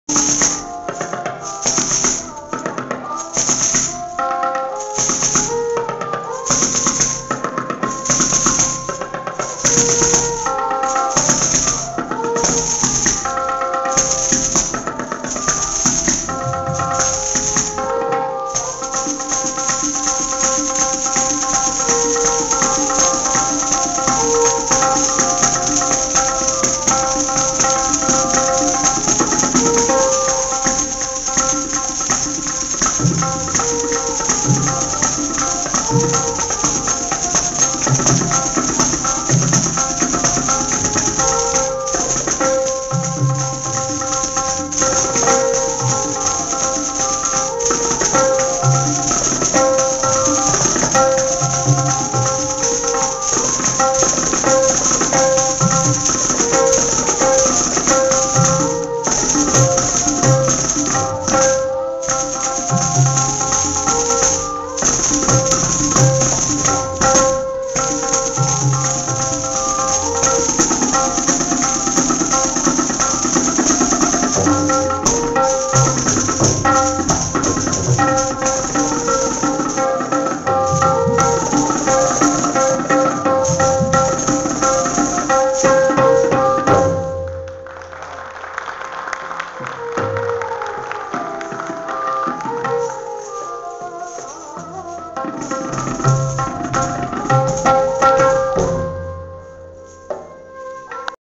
Alokparna recorded live in Calgary on Oct 10, 2009 - clip 4
Kathak - Alokparna
Sarangi - Pandit Ramesh Misra
Tabla - Subhajyoti Guha
Cocert arranged by Ragamala